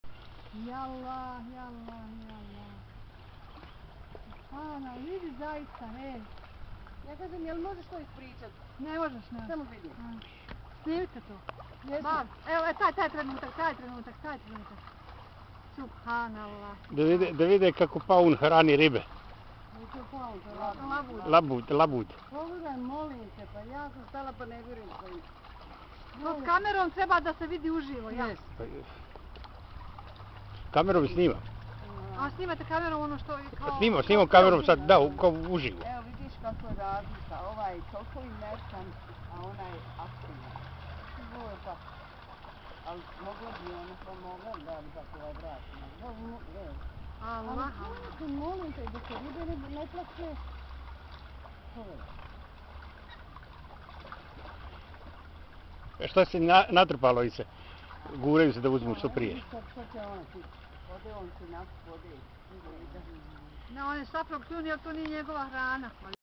Labud hrani ribe
A swan feeding bread to a hungry school of fish.